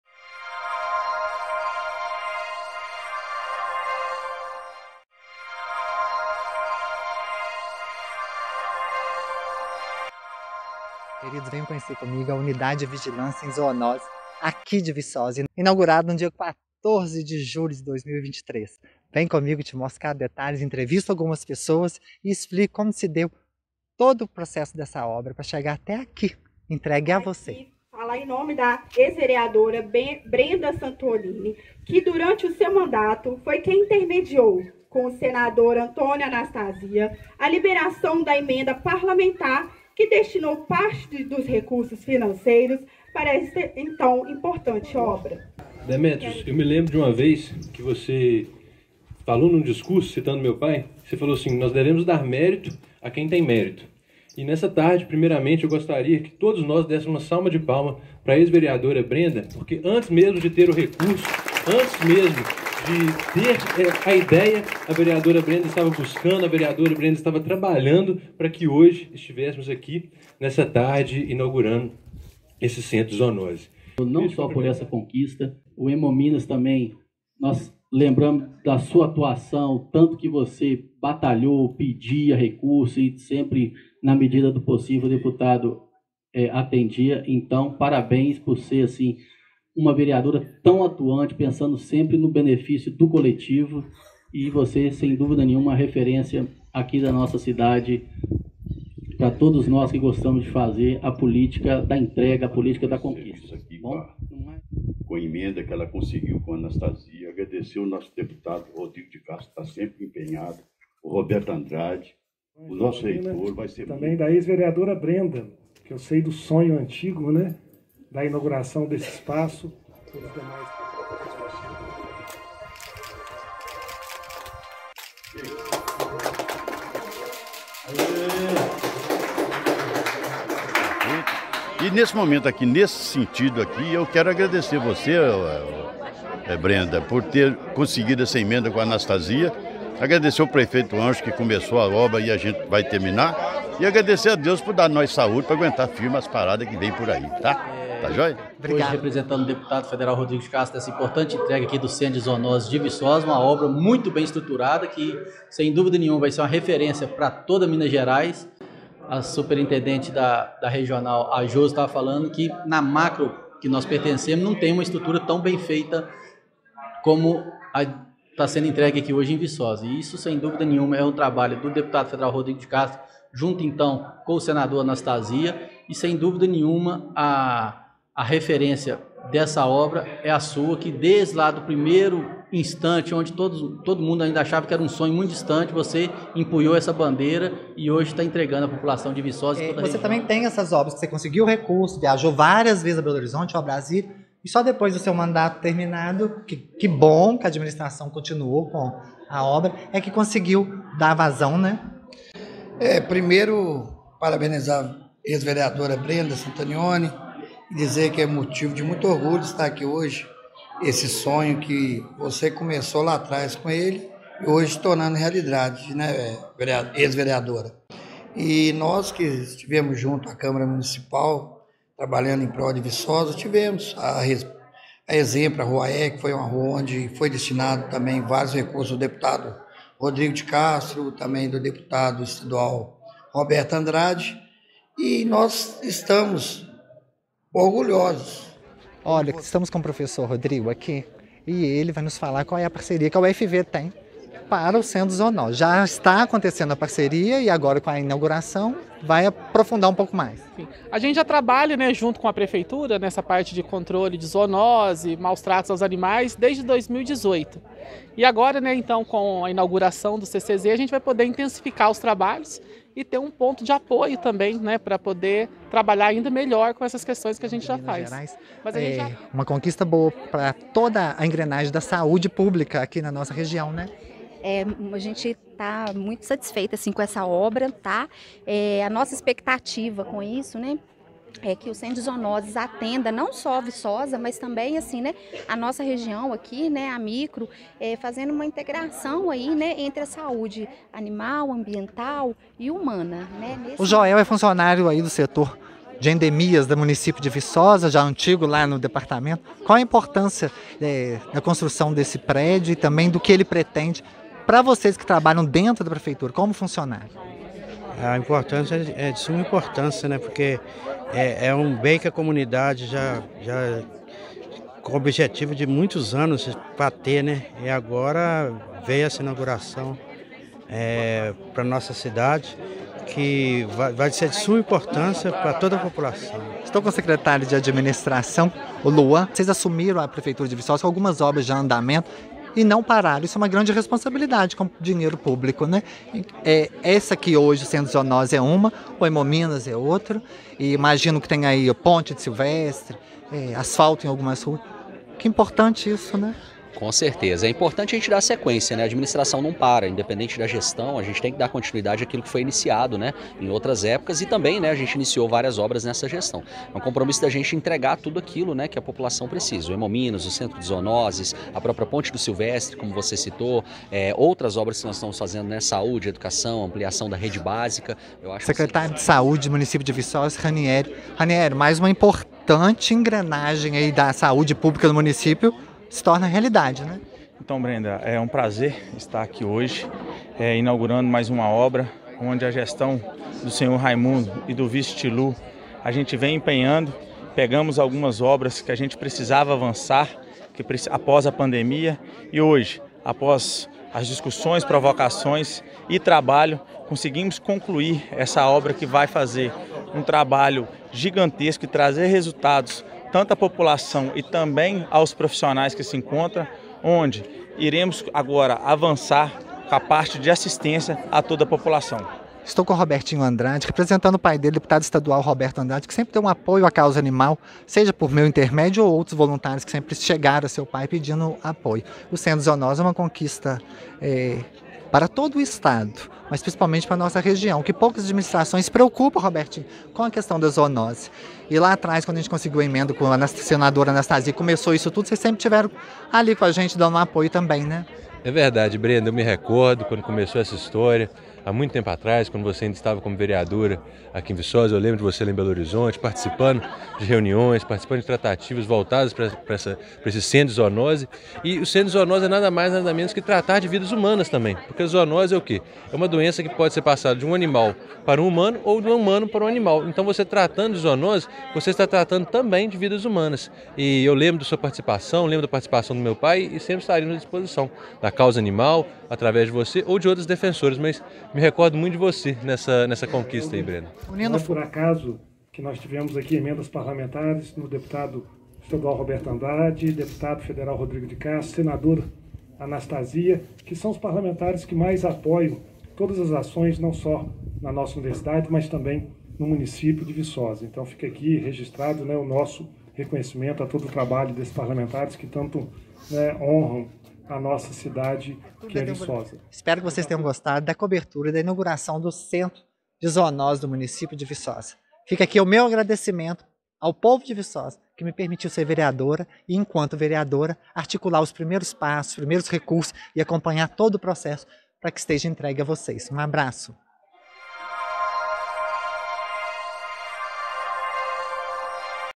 Inauguração da Unidade de Vigilância de Zoonoses em Viçosa.
Uma obra tão esperada pelo setor de saúde pública na região, em fim se torna realidade. O que se espera, a partir de agora, é que sua utilização contemple as ações de prevenção tão bem executadas pelos voluntários da causa animal, que resgatam, tratam, castram e promovem feiras de adoção para diminuir a população de animais errantes e consequentemente evitar a zoonoses.